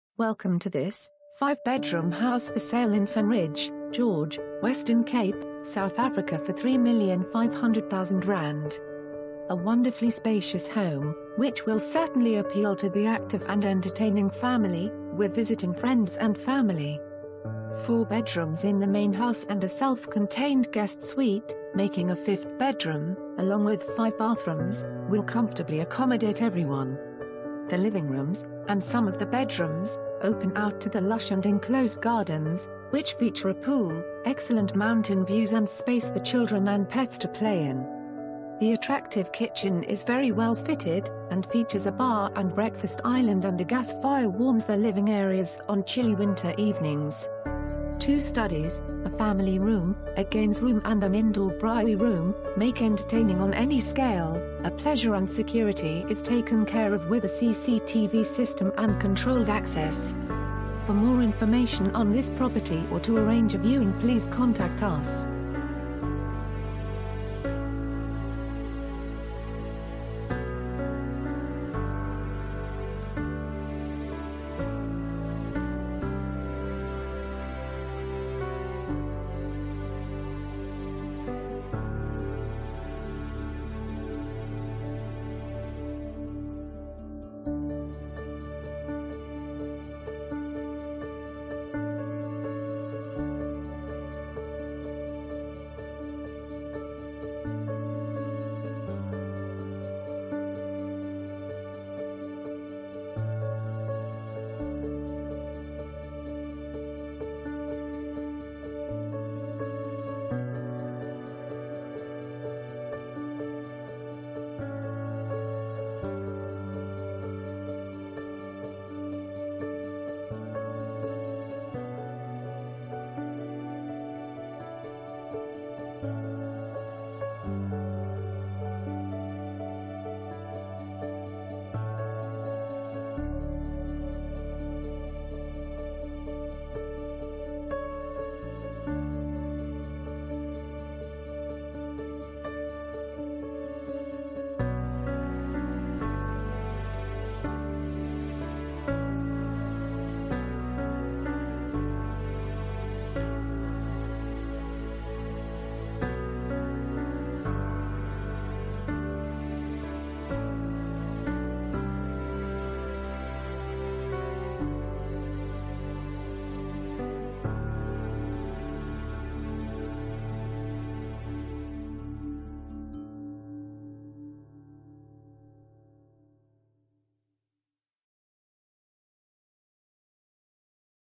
5 Bedroom House For Sale in Fernridge, George, Western Cape, South Africa for ZAR 3,500,000
A wonderfully spacious home, which will certainly appeal to the active and entertaining family, with visiting friends and family!  Four bedrooms in the main house and a self-contained guest suite, making a fifth bedroom, along with five bathrooms, will comfortably accommodate everyone.

The living rooms, and some of the bed...